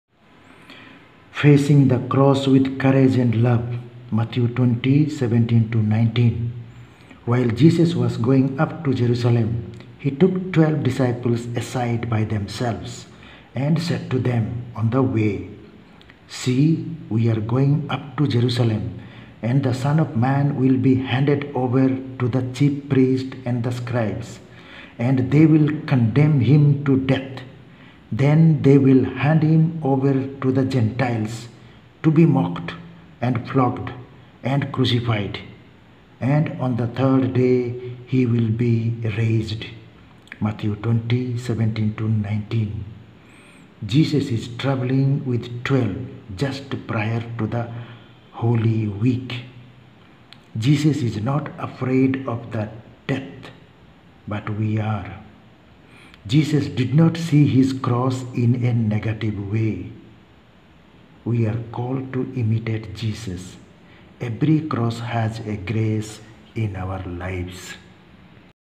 Facing the cross with courage & love (Mt 20, 17-19).
Mt 20,17-19
17 While Jesus was going up to Jerusalem, he took the twelve disciples aside by themselves, and said to them on the way, 18 “See, we are going up to Jerusalem, and the Son of Man will be handed over to the chief priests and scribes, and they will condemn him to death; 19 then they will hand him over to the Gentiles to be mocked and flogged and crucified; and on the third day he will be raised.”

Hello Friends,